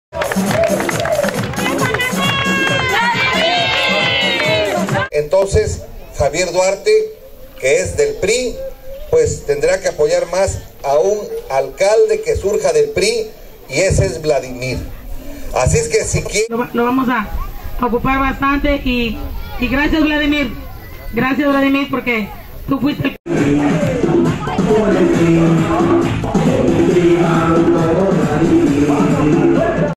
Héctor Yunes Landa visita Villa Azueta...No baja la guardia.
JOSE AZUETA, VER., 20 DE MAYO DE 2011.- El presidente del Comité Directivo Estatal del Partido Revolucionario Institucional, Héctor Yunes Landa, estuvo en Dobladero, Cujuliapan y en esta cabecera municipal, para apoyar la campaña del candidato a la alcaldía, Vladimir Auli Pérez. Dialogó con cientos de militantes y simpatizantes de distintos sectores del PRI.